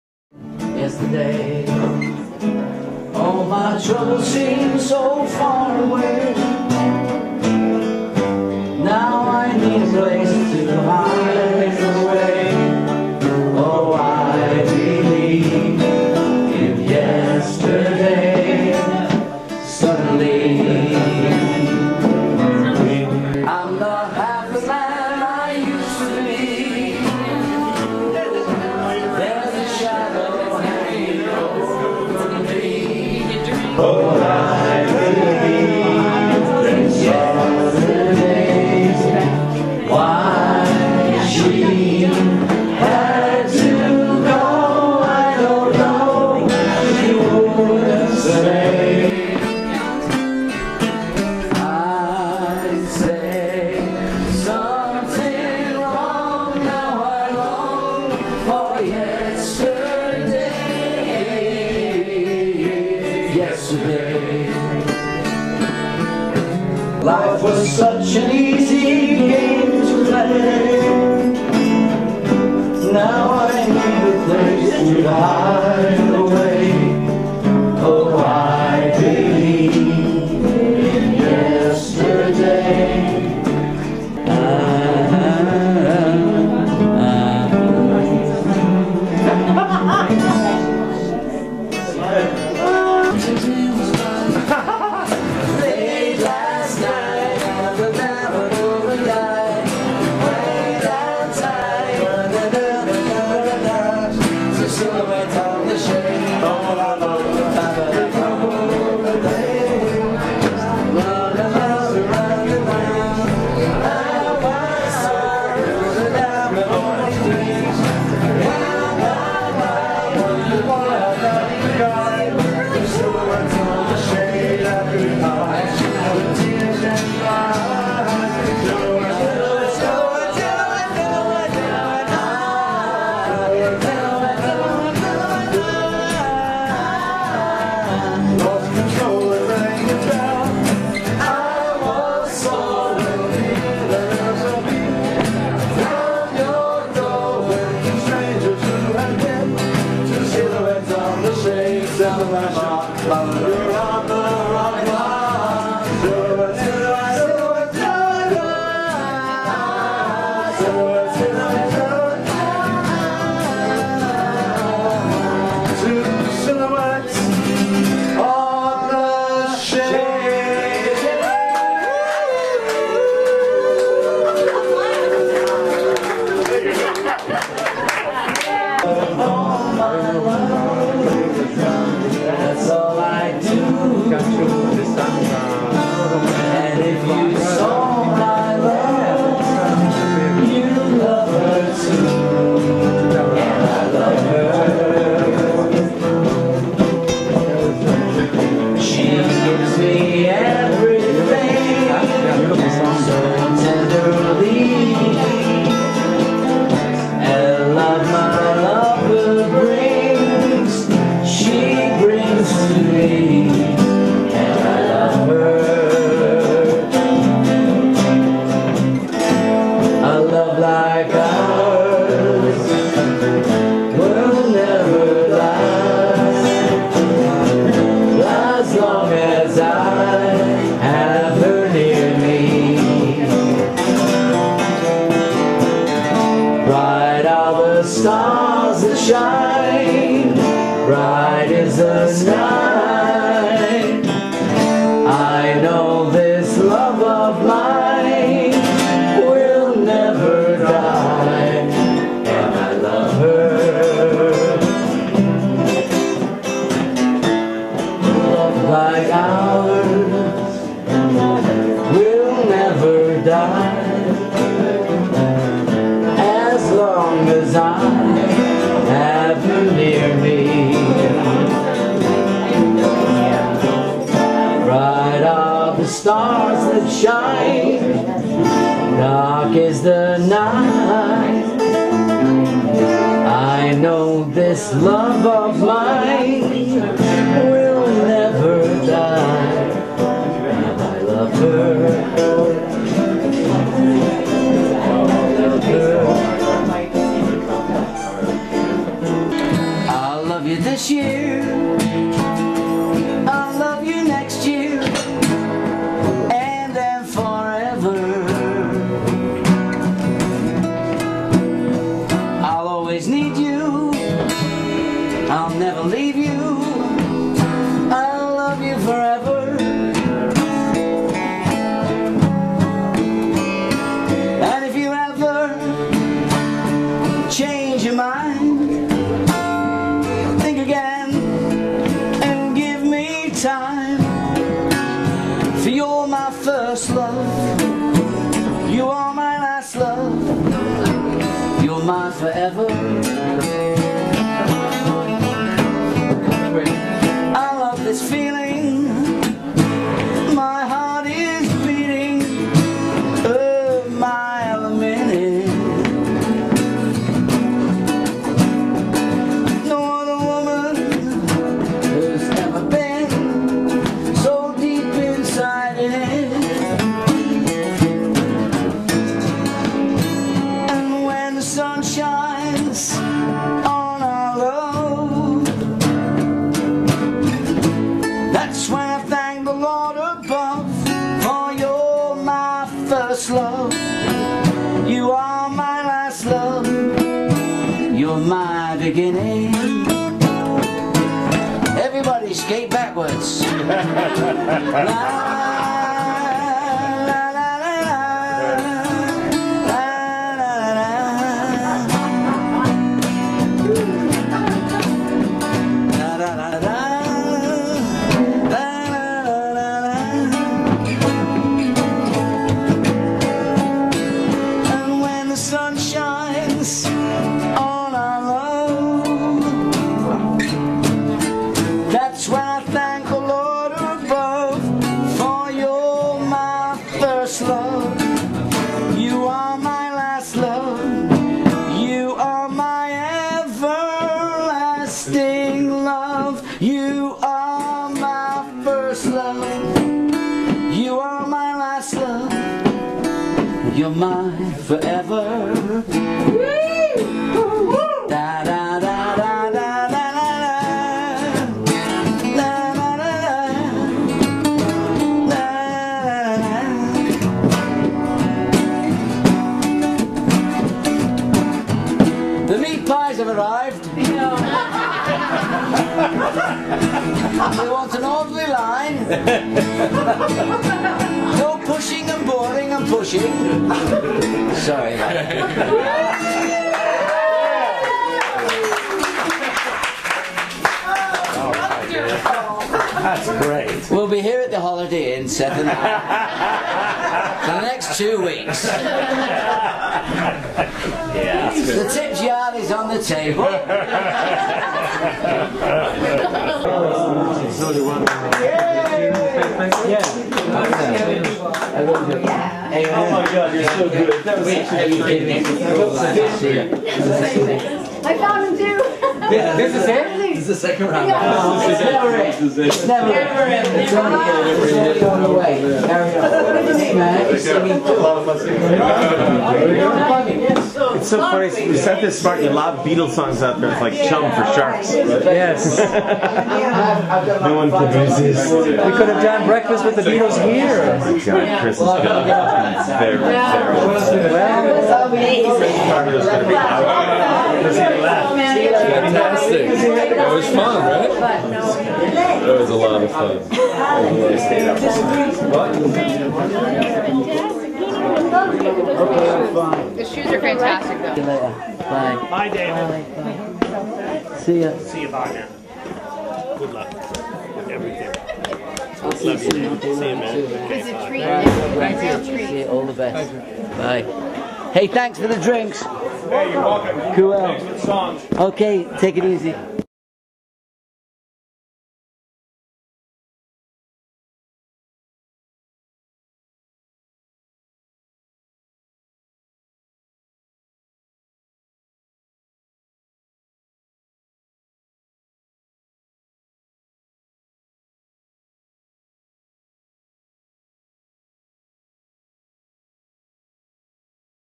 Davy Jones singalong at Seth Swirsky's birthday party (part 3)
Davy Jones, of The Monkees, in a singalong at Seth Swirsky's birthday party. Pt. 3  (8/7/10)